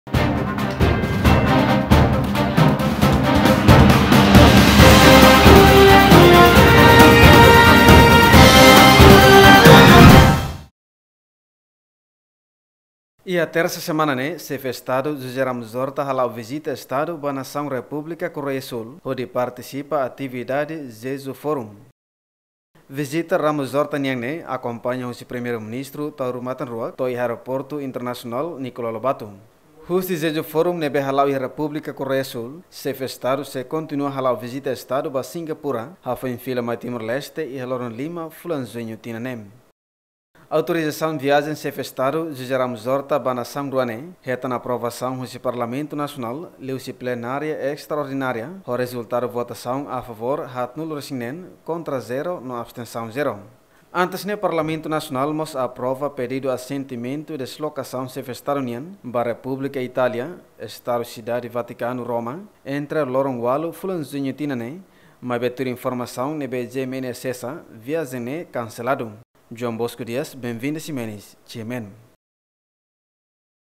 PR Horta hala’o vizita Estadu ba Korea Do Súl no Singapura
Prezidente Repúblika José Ramos Horta hala’o vizita Estadu ba Korea Do Súl no Singapura